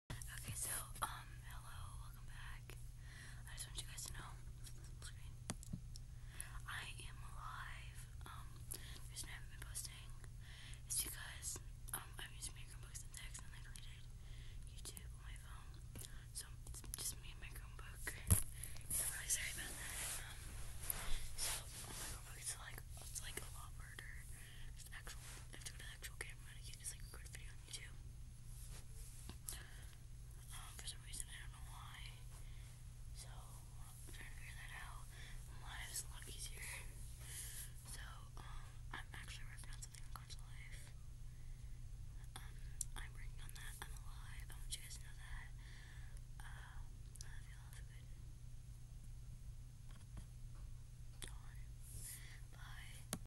IM ALIVE (read desc)
im alive and i want you guys to know thati love you and im trying to post more videos but its hard because i have IOWA Testing soon